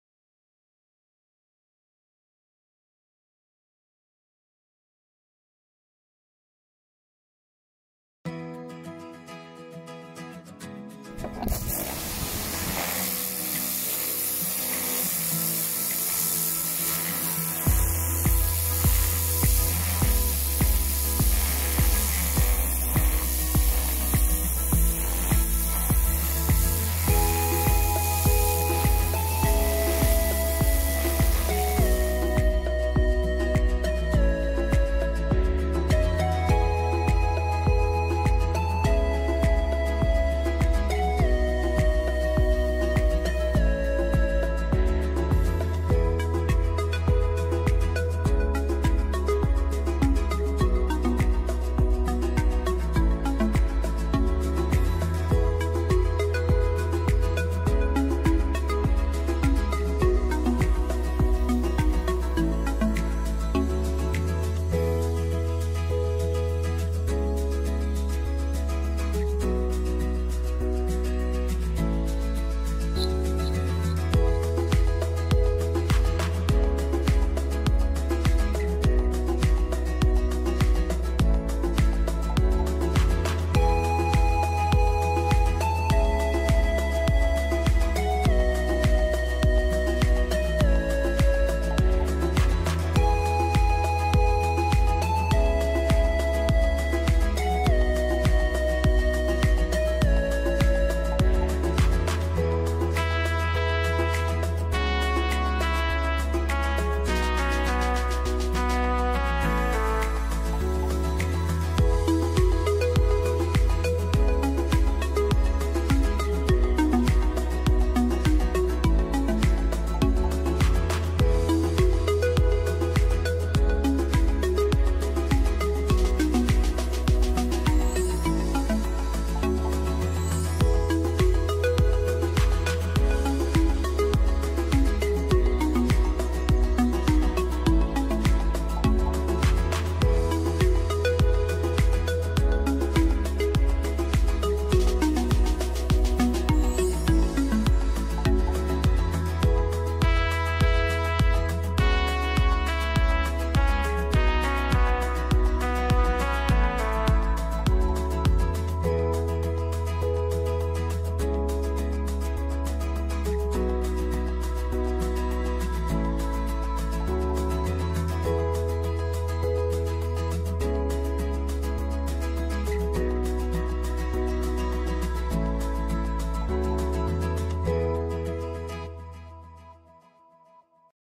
Tejado impermeabilizado con Poliurea, Polinova Global S.L
Impermeabilización y protección de tejado de teja plana de cemento con Poliurea. Gracias por confiar en nosotros.